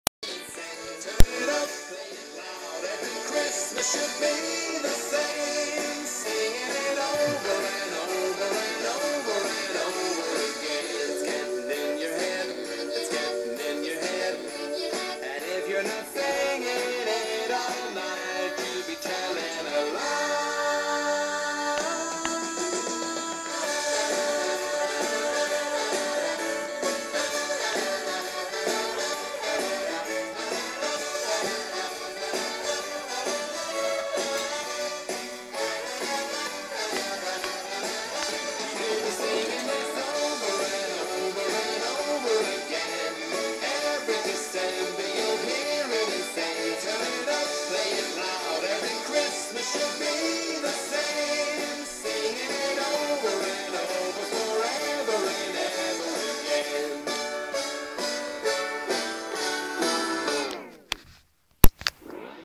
MEEZER CHRISTMAS FUN
MARSHALL AND LOLLY HAVING A CHRISTMAS PLAY TIME